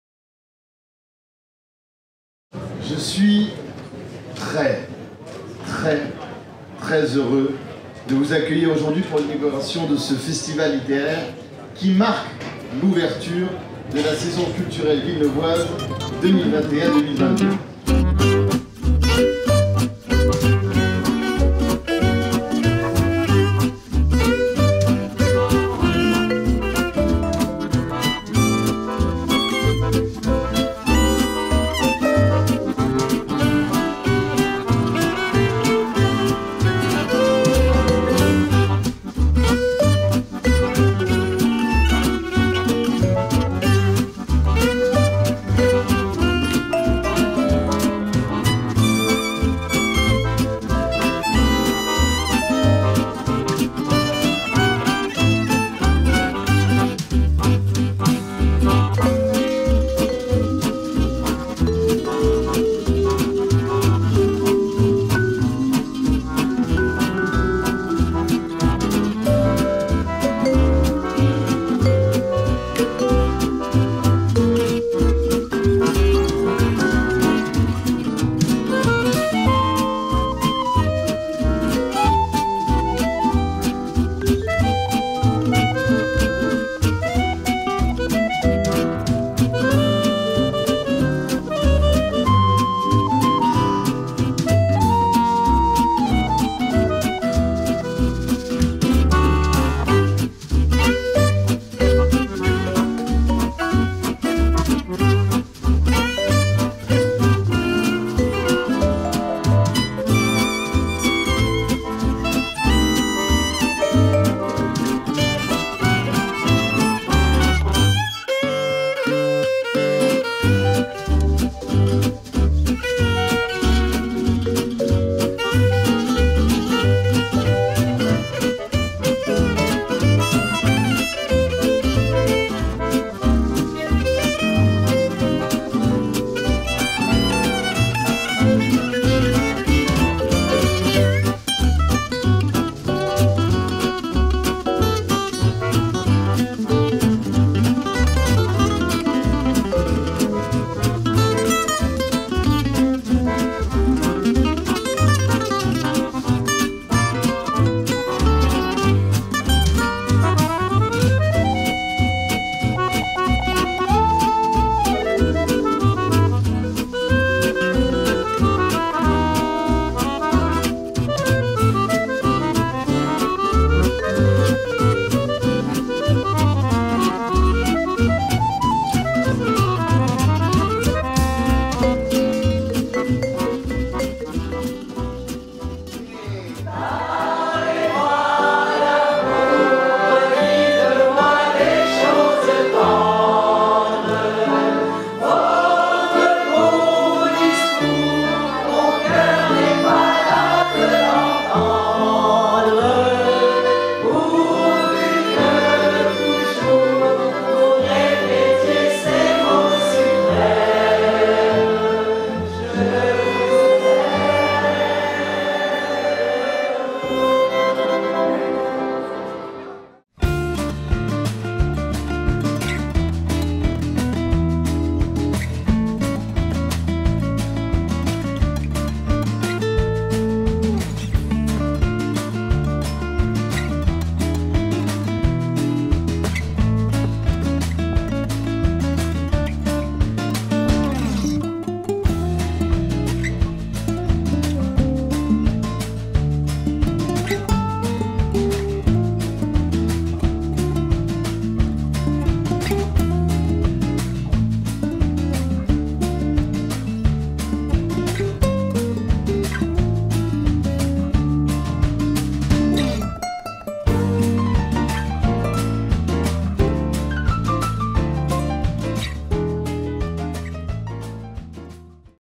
Festival littéraire "Villeneuve se livre" 2021
Retour en images sur l'édition 2021 du festival littéraire "Villeneuve se livre" de Villeneuve-sur-Lot